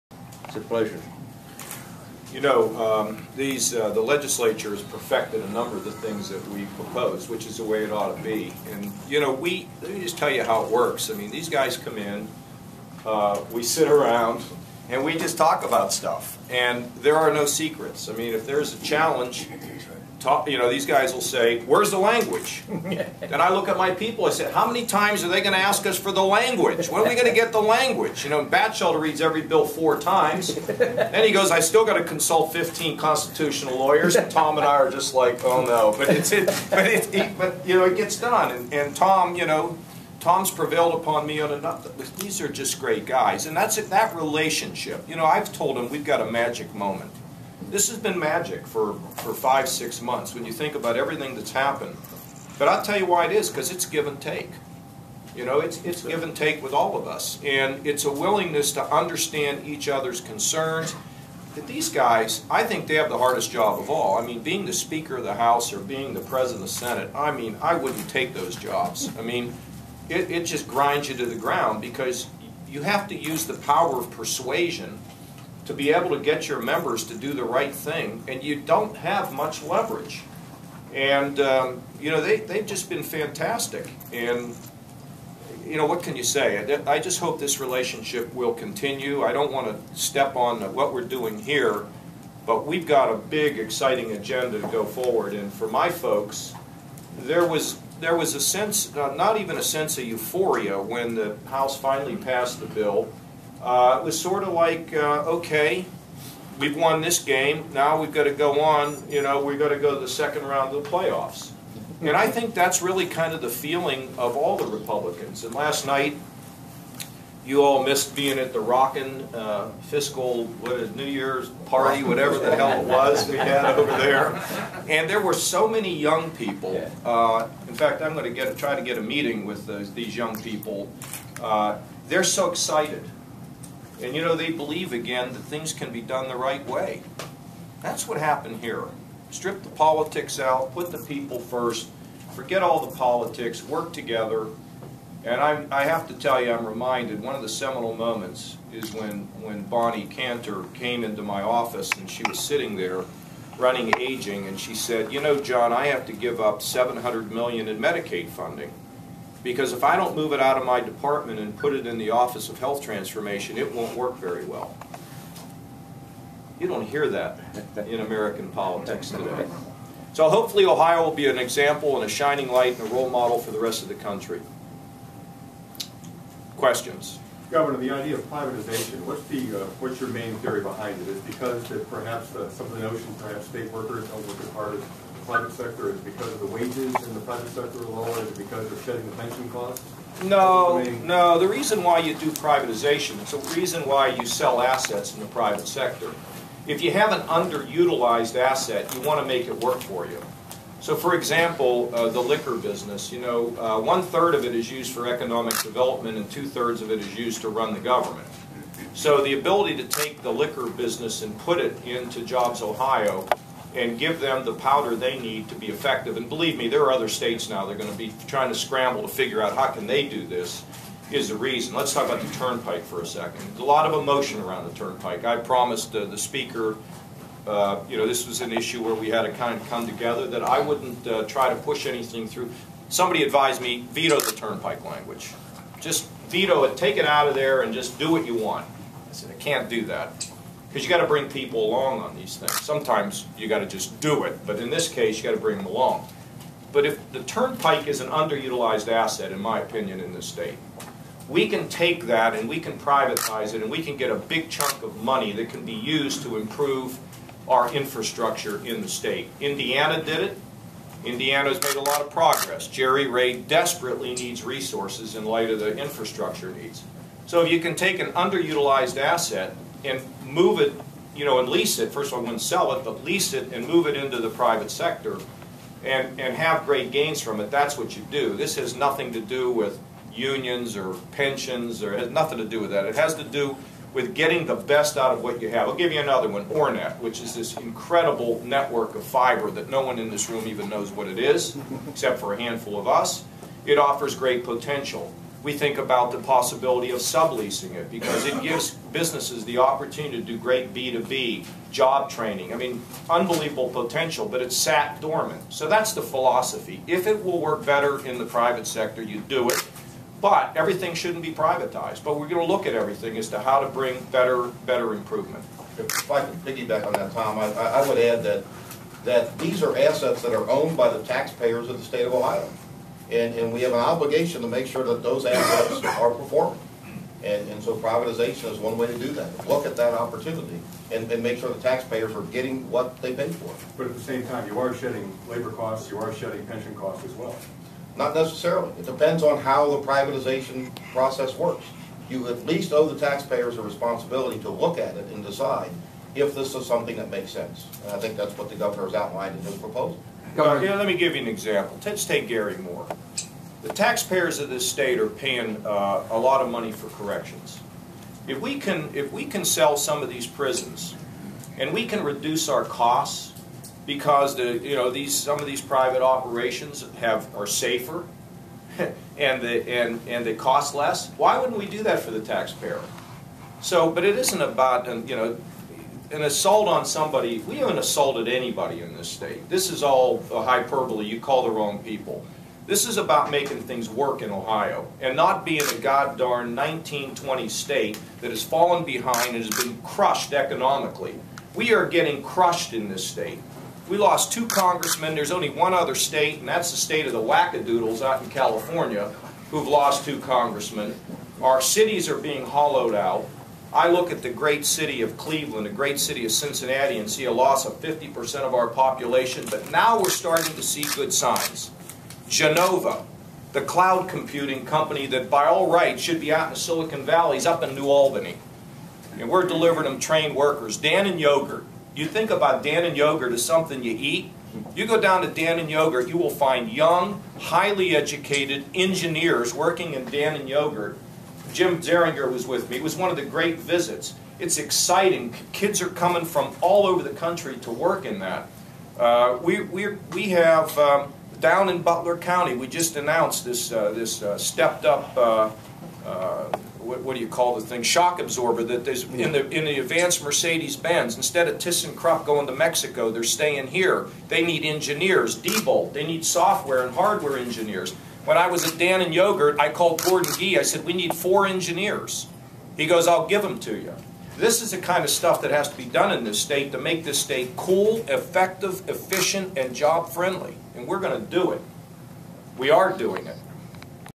July 1 Kasich Budget Q and A Part 1.mov
Ohio Gov. John Kasich, Senate President Tom Niehaus and House Speaker Bill Batchelder discuss the $56 billion biennial budget during a press conference at the governor's residence the day after Kasich signed the legislation into law.